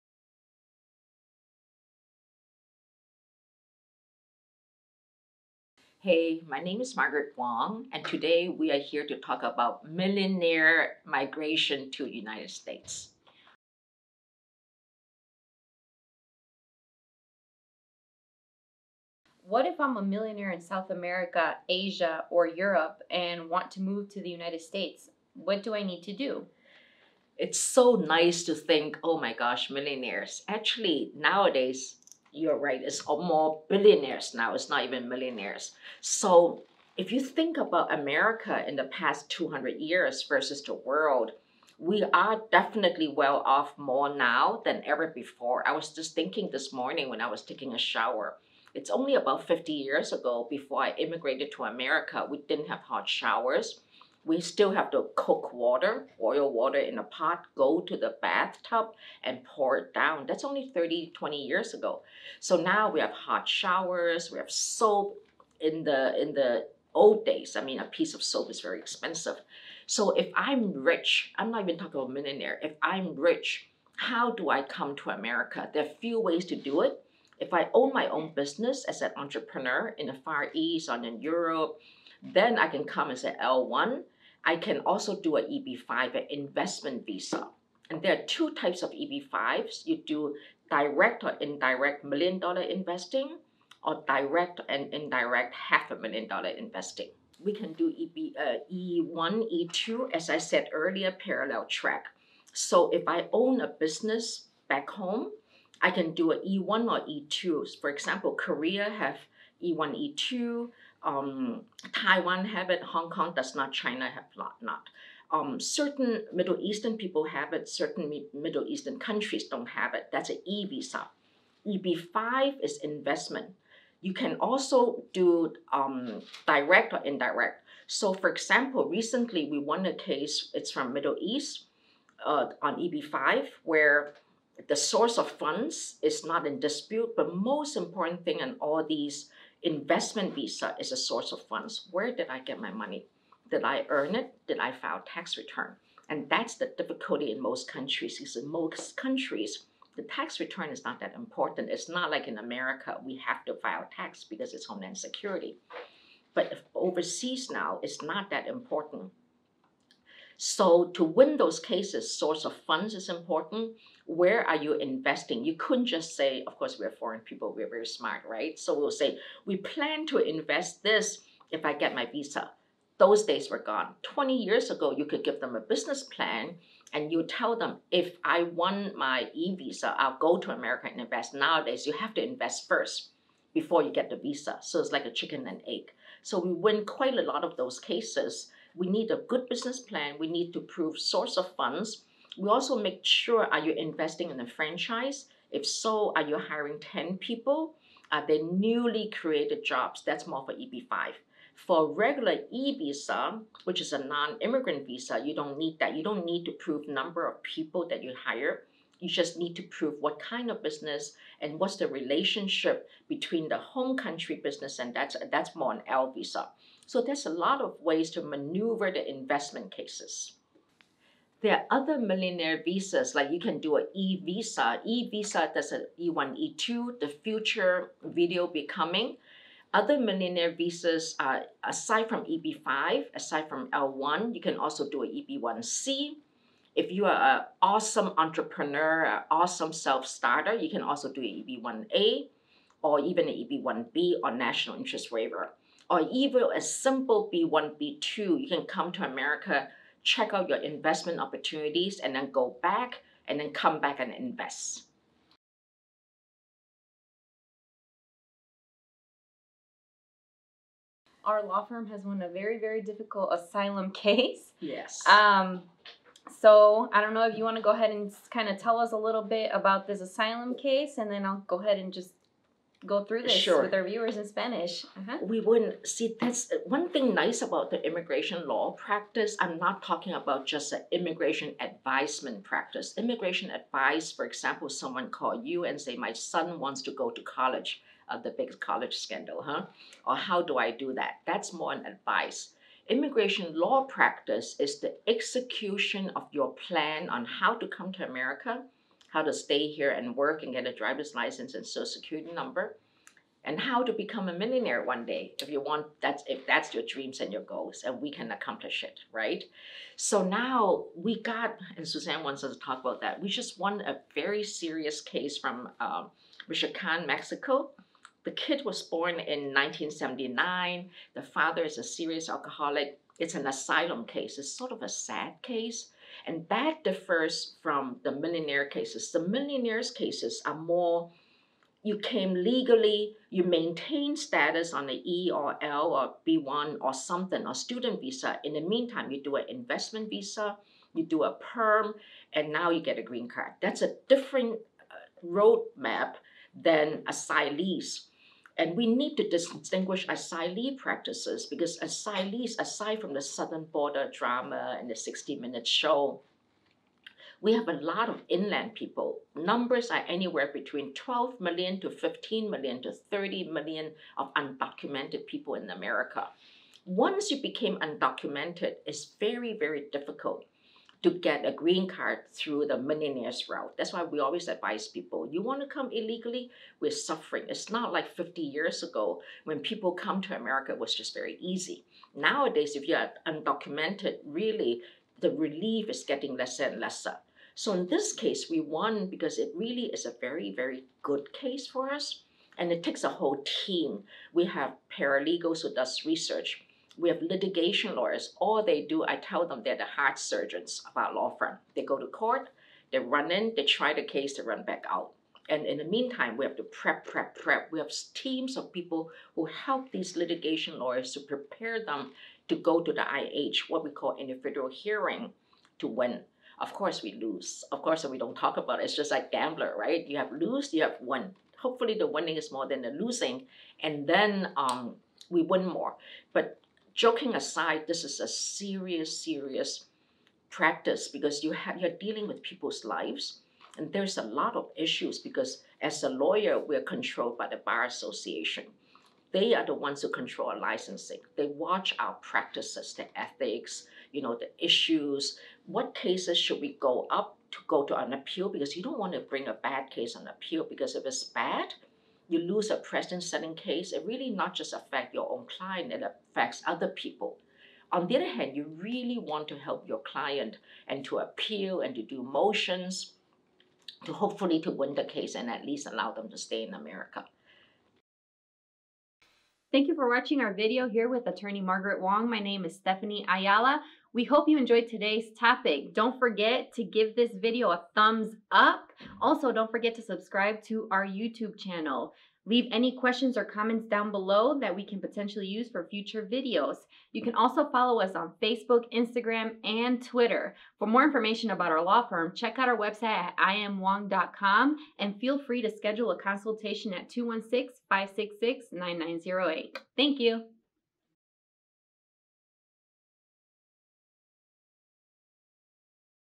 How do millionaires move to America? | America's Best Immigration Attorneys? | Margaret W. Wong
If I am a millionaire in South America, Asia or Europe and want to move to the US. What do I need to do? Managing Partner, Margaret W. Wong explains, and shares a recent success story!